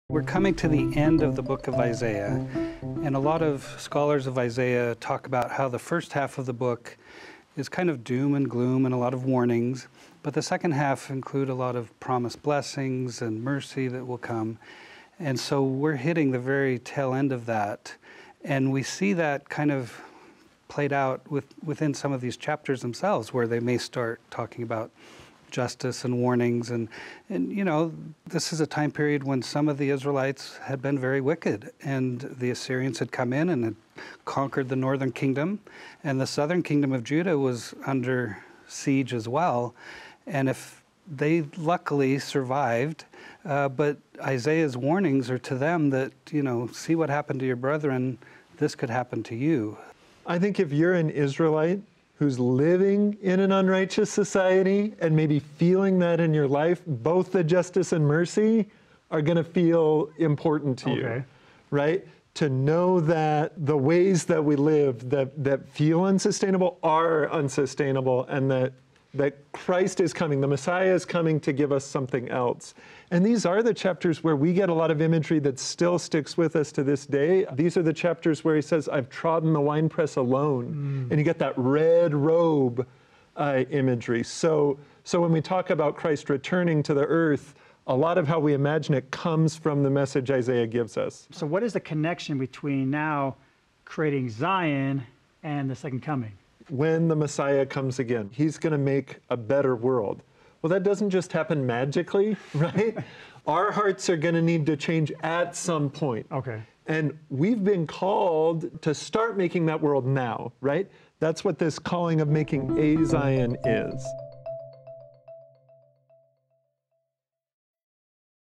Justice and Mercy in Isaiah
James Goldberg and Jared Ludlow talk about the language of both justice and mercy in Isaiah.